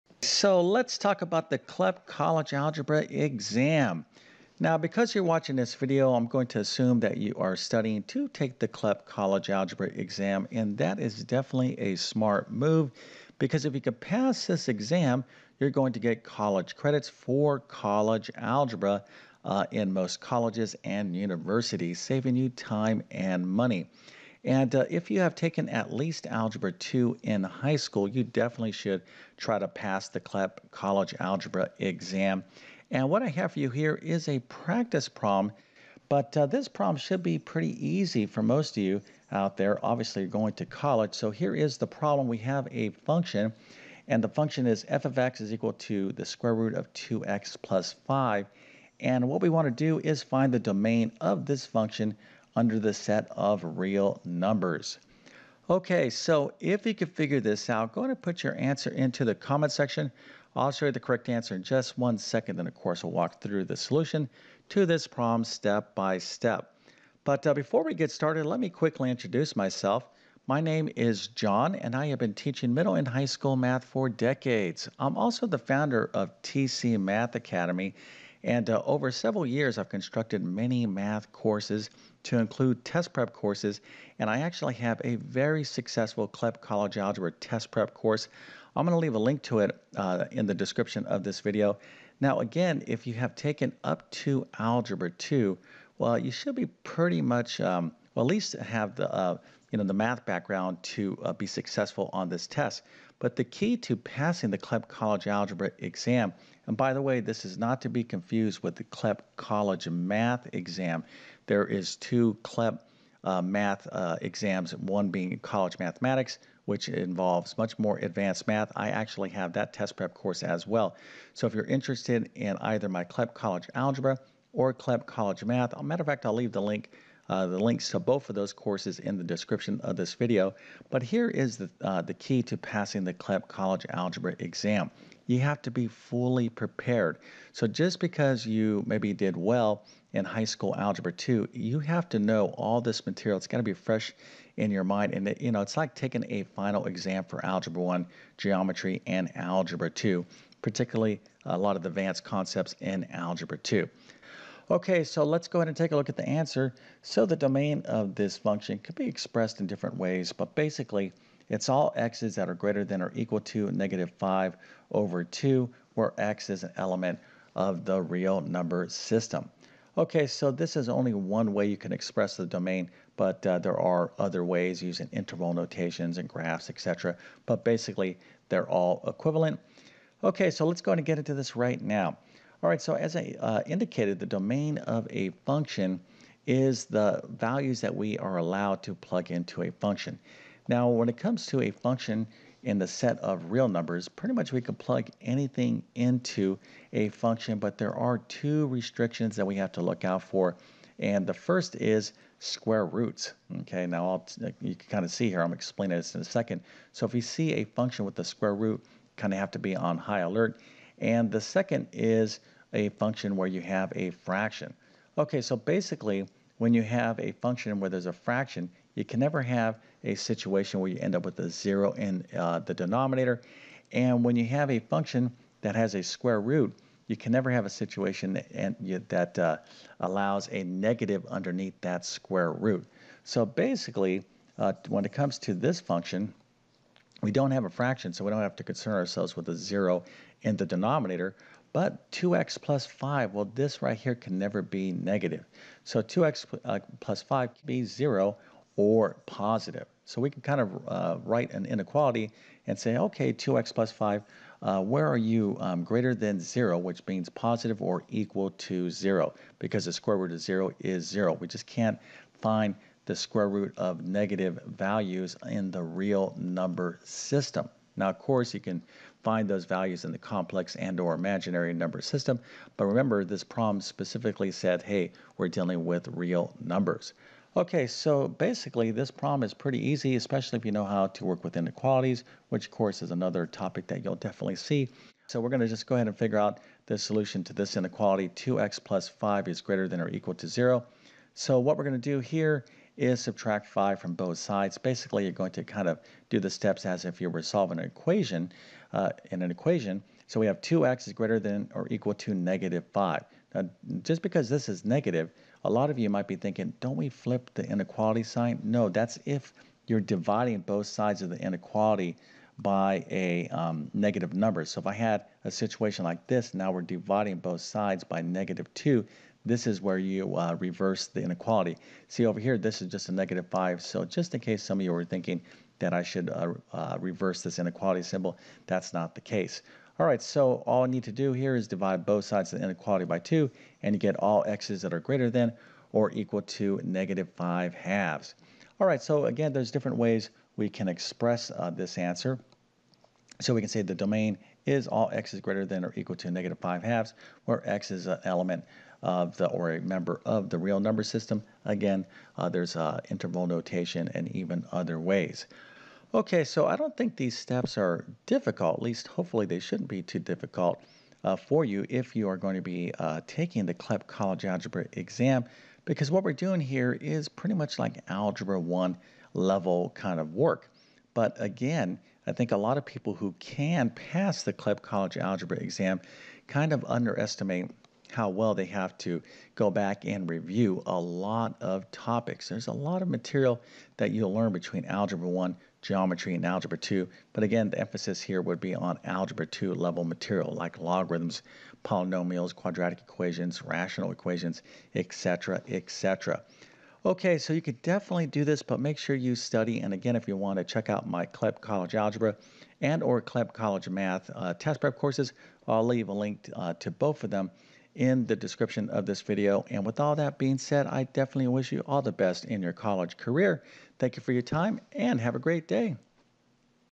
CLEP College Algebra – How To Pass In 2025!
CLEP College Algebra – Practice Problem: Powers and Exponents

Pass the CLEP College Algebra Exam and you will have taken advantage of a great opportunity to earn college credit.  In this video I will go over a practice problem on powers and exponents - a math skill you will need to know to pass the CLEP College Algebra exam. If you need math help in passing the CLEP College Algebra Exam check out my CLEP College Algebra Math Test Prep Course: